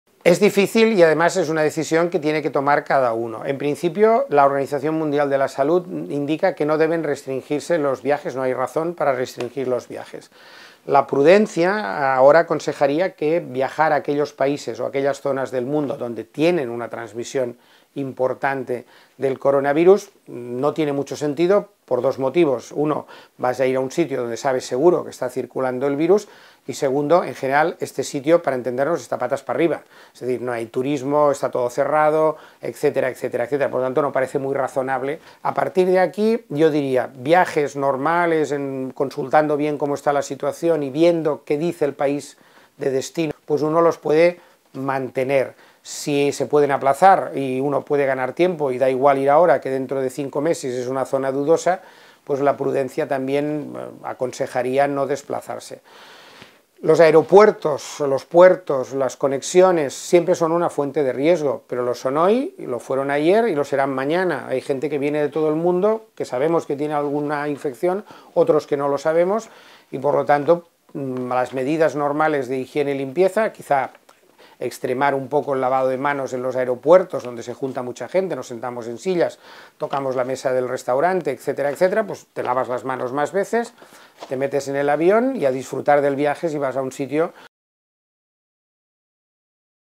¿Es arriesgado viajar durante el Coronavirus? | PortalCLÍNIC
El Dr. Antoni Trilla, jefe de Medicina Preventiva y Epidemiología del Hospital Clínic, las recomendaciones sobre viajar en el momento actual.